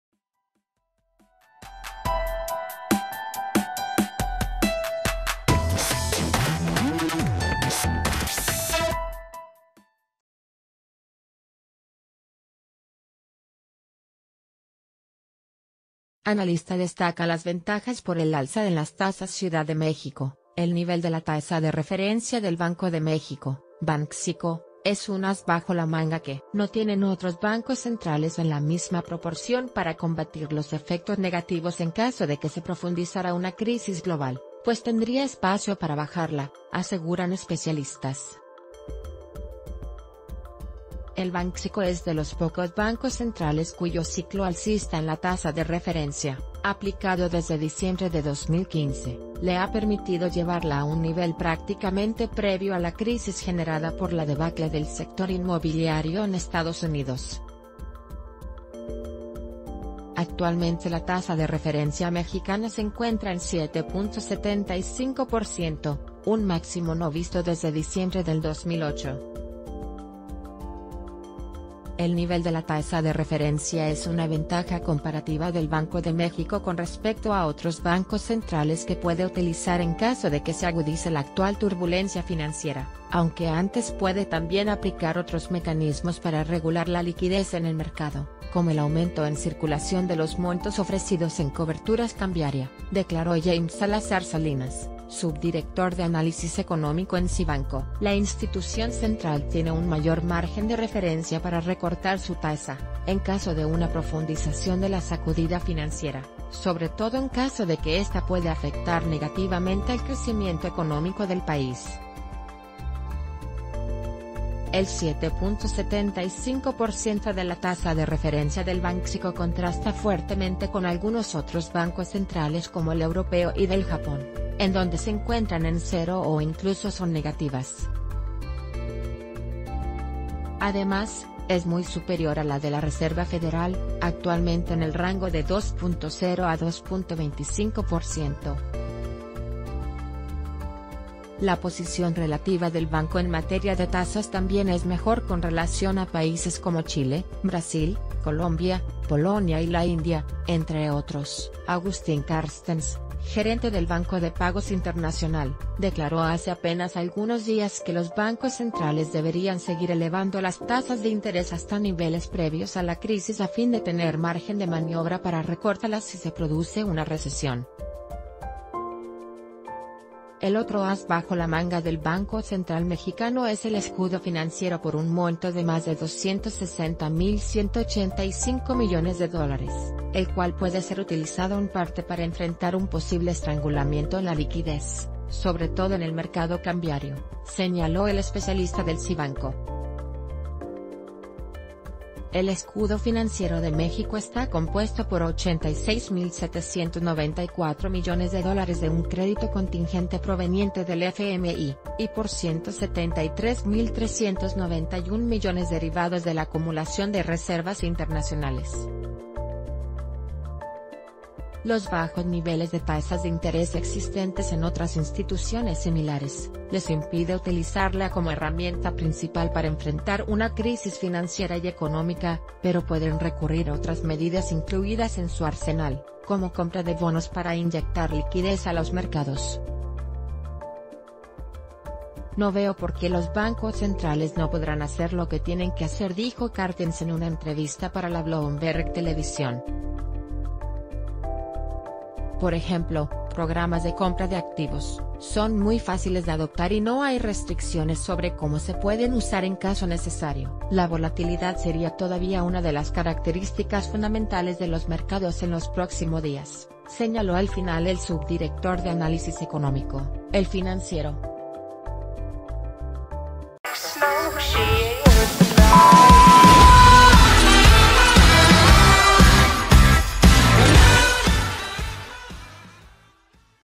El “as bajo la manga” del Banxico - El Diario de Yucatán
Analista destaca las ventajas por el alza en las tasas CIUDAD DE MÉXICO.— El nivel de la tasa de referencia del Banco de México (Banxico) es un “as bajo la manga” que no tienen otros bancos centrales en la misma proporción para combatir los efectos negativos en caso de que se profundizara una crisis...